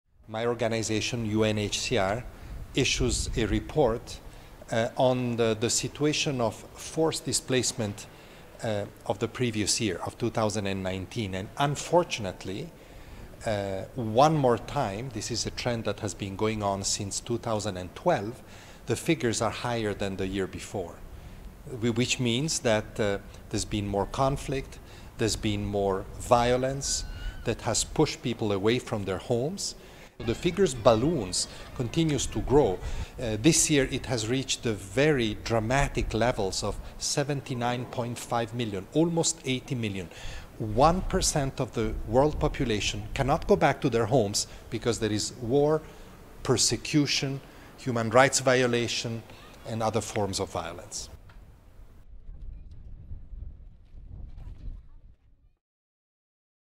Over 1% of humanity displaced, says UN's Filippo Grandi | AFP
Nearly 80 million people have been forced to flee their homes due to violence and persecution, marking a near-doubling of global displacement in a decade, says UNHCR chief Filippo Grandi.